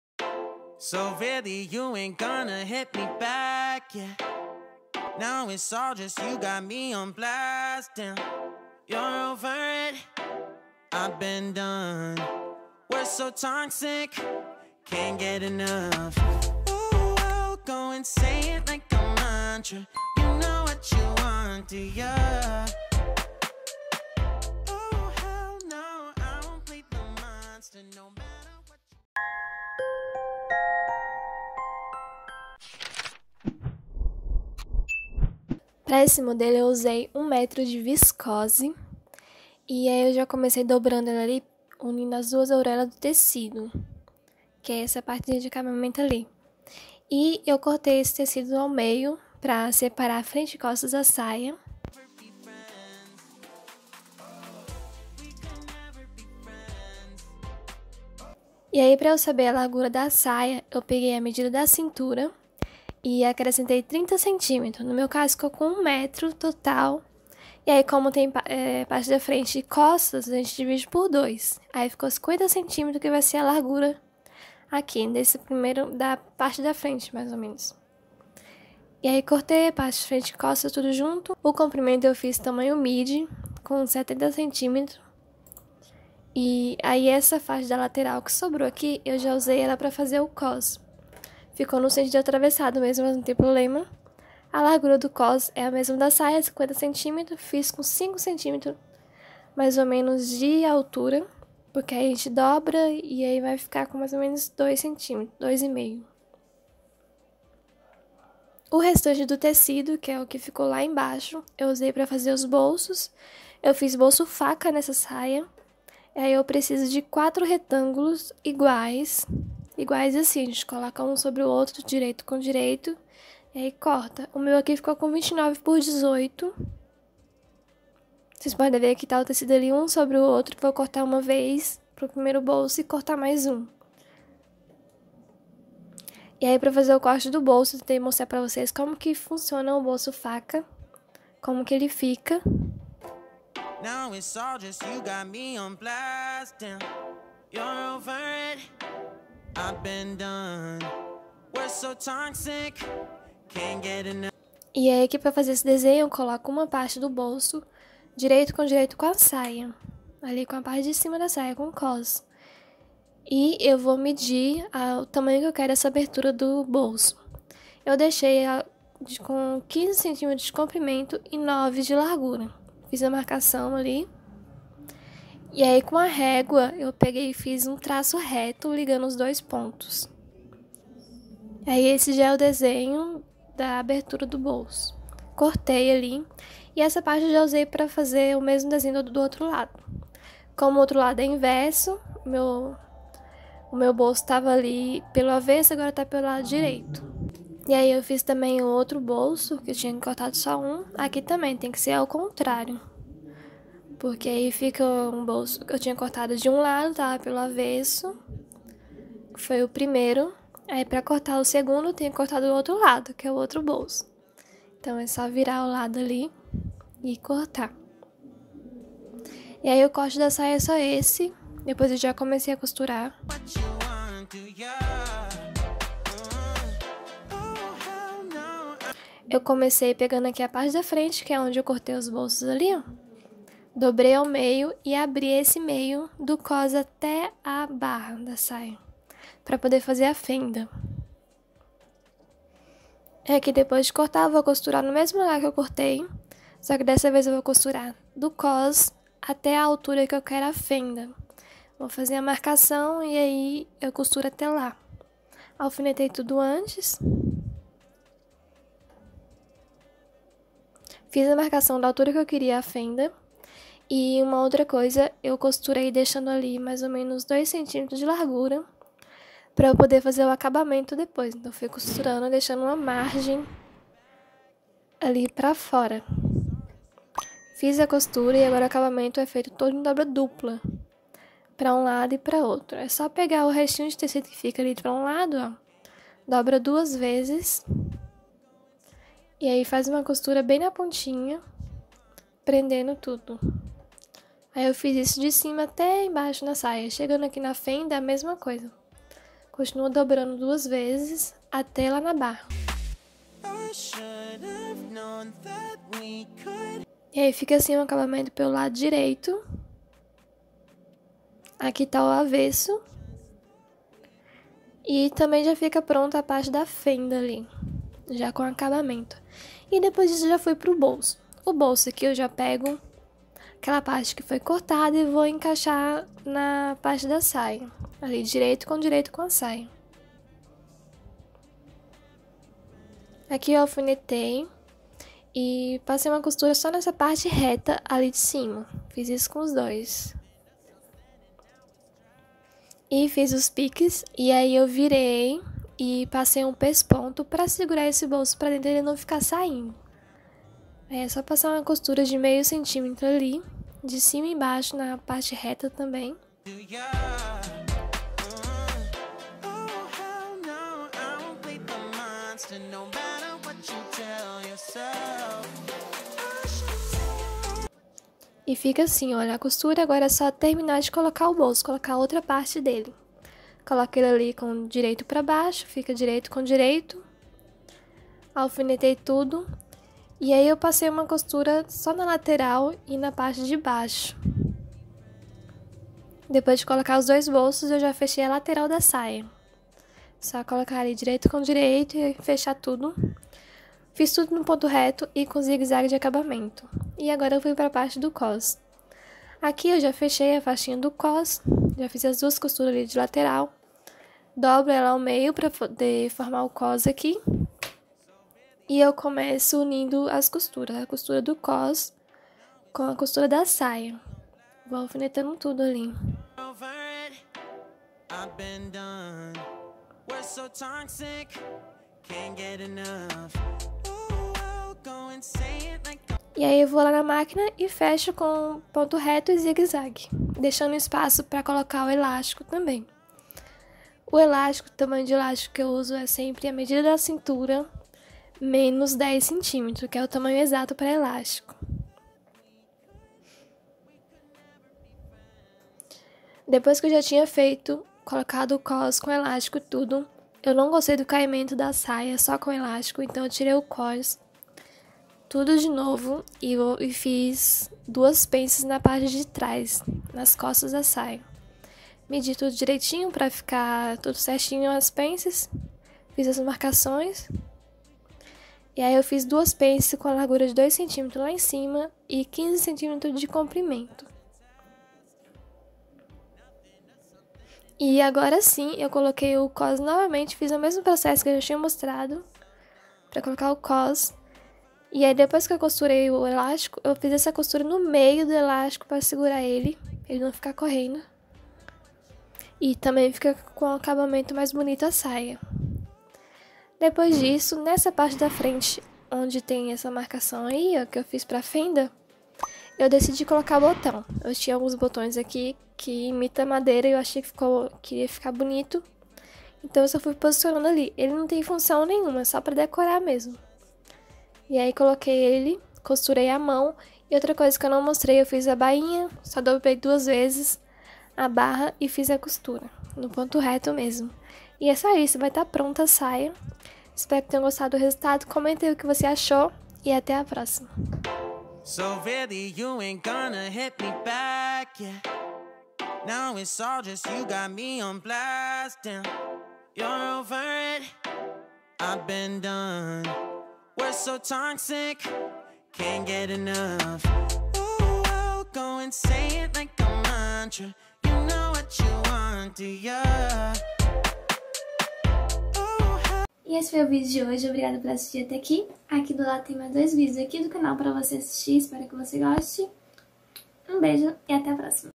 DIY: SAIA MIDI / COMO FAZER BOLSO FACA
Tecido: Viscose
Quant. para tamanho P: 1 metro
Agulha: nº 11

EDITOR USADO: FILMORA WONDERSHARE